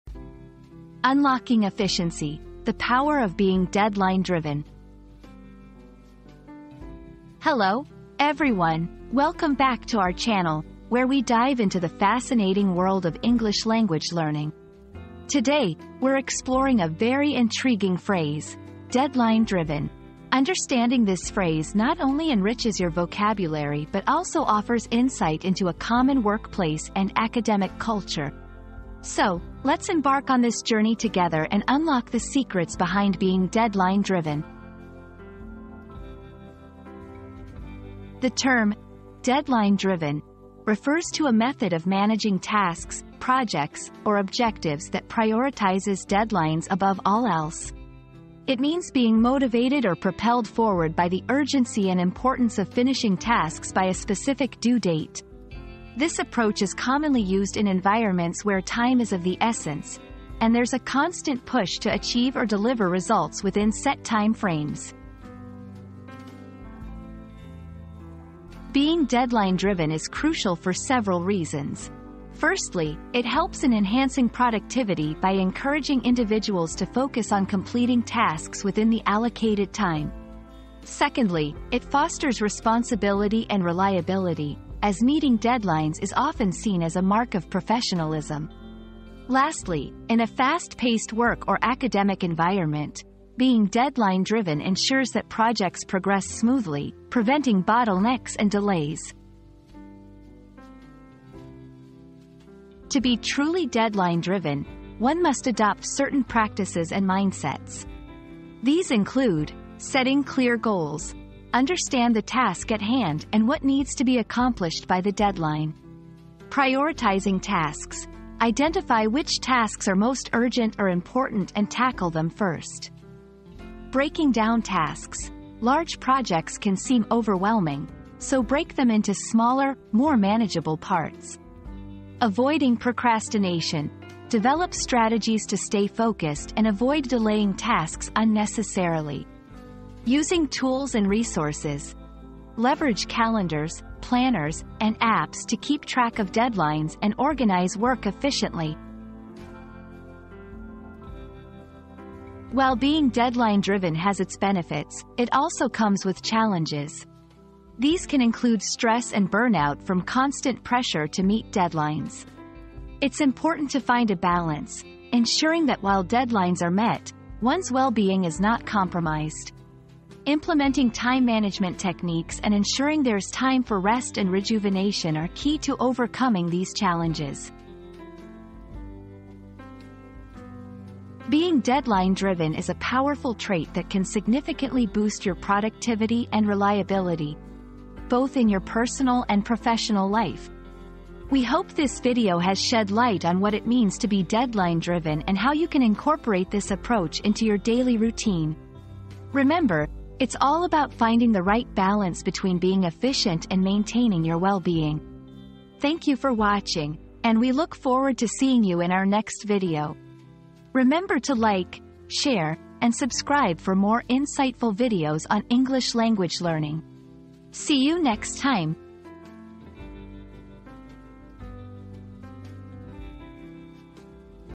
Unlocking Efficiency: The Power of Being Deadline-Driven
00:00 • Introduction - Unlocking Efficiency: The Power of Being Deadline-Driven
00:37 • What Does "Deadline-driven" Mean?
01:08 • The Importance of Being Deadline-driven
01:42 • How to Be Deadline-driven
02:31 • The Challenges of Being Deadline-driven